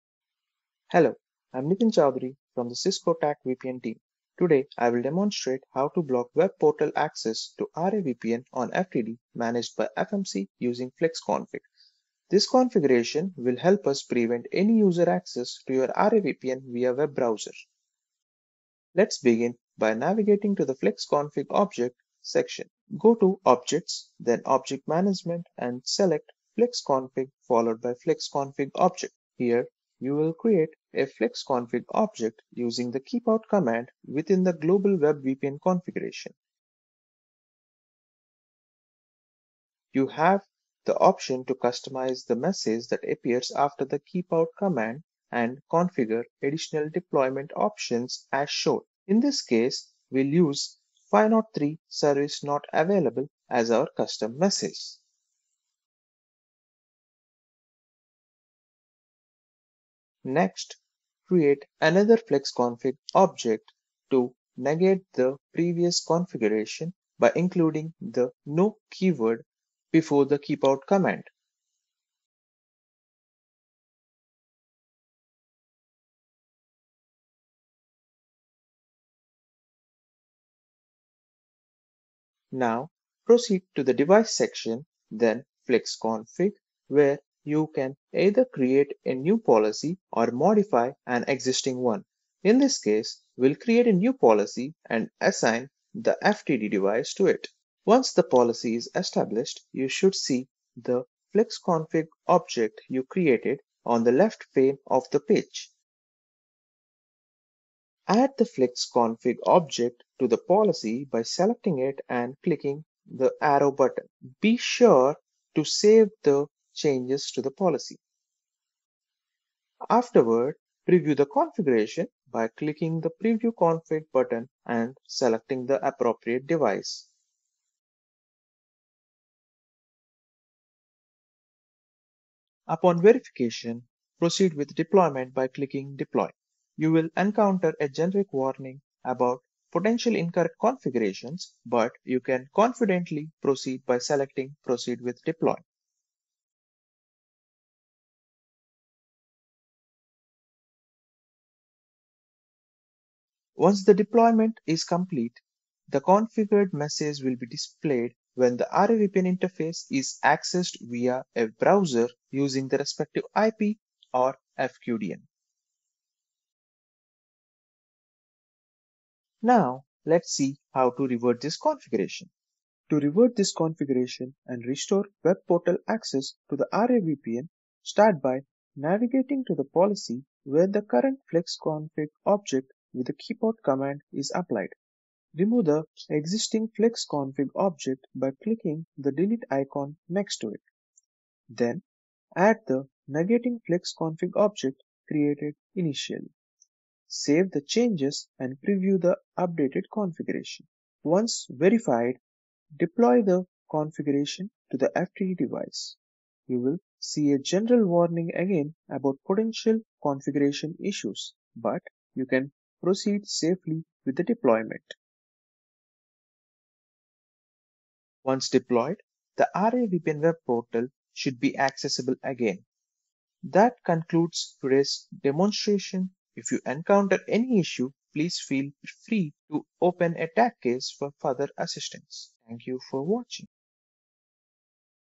How to Block Web Portal Access to Remote Access VPN on Cisco FTD via FMC
Learn how to block web portal access for Remote Access VPN (RAVPN) users on Cisco Secure Firewall Threat Defense (FTD) managed by Firepower Management Center (FMC). This video walks you through the configuration process to ensure VPN access is restricted to client-based connections only—boosting control and minimizing exposure.

  What you’ll learn in this video:
  – How to disable web portal access for RAVPN on Cisco FTD
  – Configuration steps using Firepower Management Center (FMC)
  – Best practices to enforce client-only VPN access and strengthen security